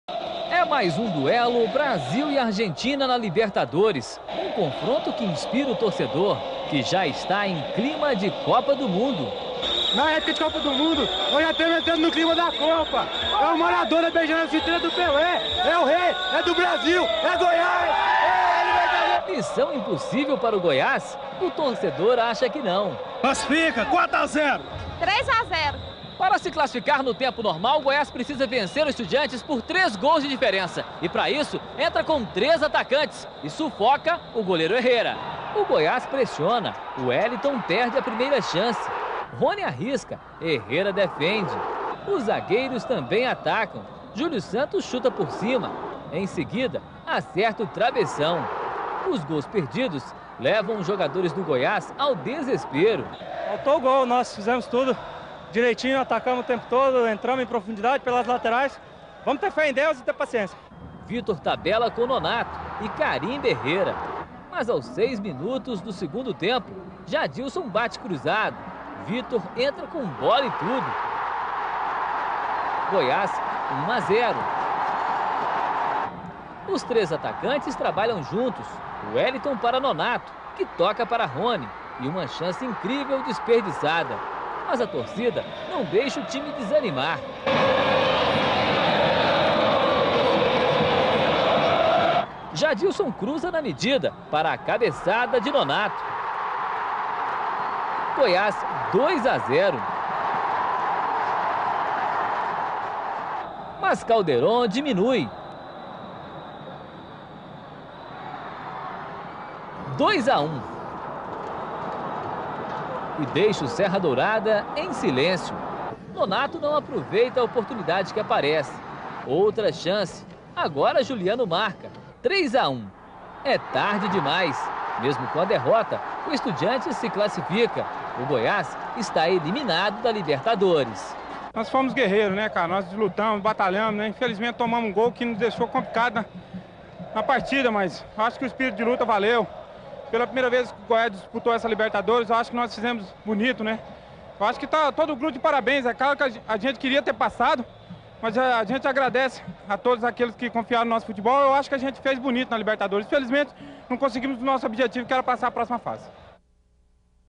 Goiás 3 x 1 Estudiantes - Copa Libertadores da América 2006
Estádio: Serra Dourada - Goiânia-GO
Gols:
1° Vítor 51'/2 (Goiás)
2° Nonato 65'/2 (Goiás)
3° José Luis Calderón 76'/2 (Estudiantes)
4° Juliano 90'/2 (Goiás)